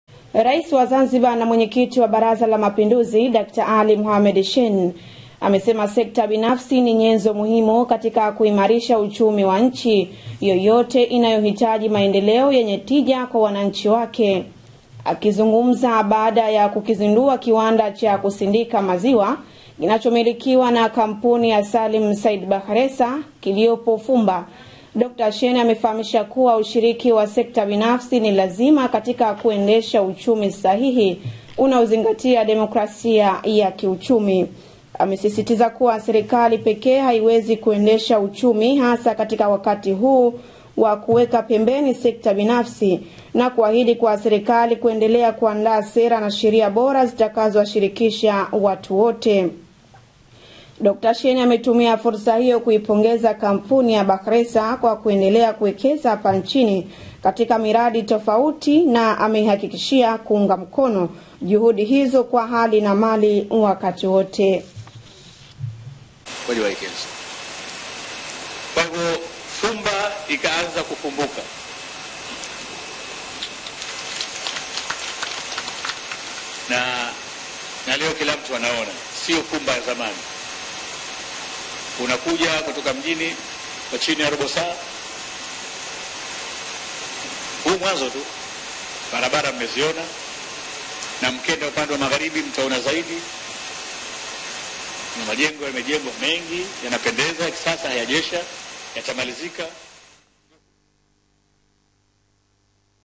Rais Shein Azipa 5 Sekta Binafsi
Rais wa Zanzibar Dkt. Shein amesema sekta binafsi ni nyenzo muhimu katika kukuza na kuboresha uchumi wa nchi